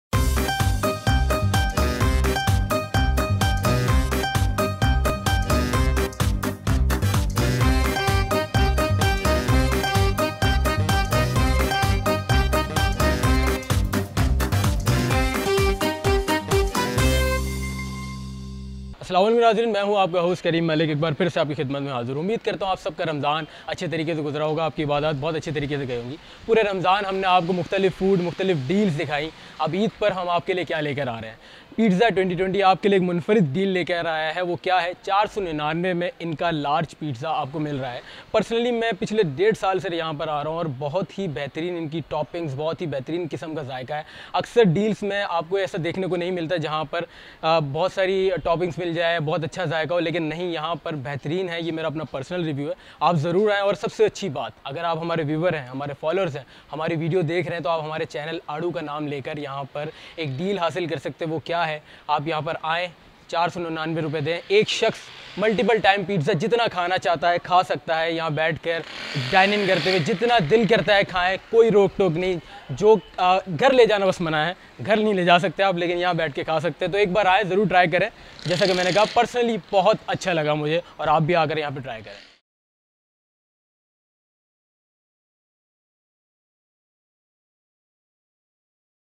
Eid Best Deal | Eat unlimited pizza for only 499/-
کراچی میں مختلف اور منفرد ڈیل دینے والا ریسٹورنٹ Pizza 2020 اب کی بار عید ڈیل میں دے رہا ہے لارج پیزا وہ بھی صرف -/499 میں...
 اور اس سے بھی  بہترین اوفر Aaro کے دیکھنے والوں کے لیے  -/499 میں ایک شخص جتنا چاہے  اتنا پیزا کھائے۔
Location:
Branch 1
Noman Center Block 5, Gulshan-e-Iqbal, Karachi.
Branch 2
Shop no. 1 Darus Salam appartment block M, North Nazimabad, Karachi.
For online orders and details:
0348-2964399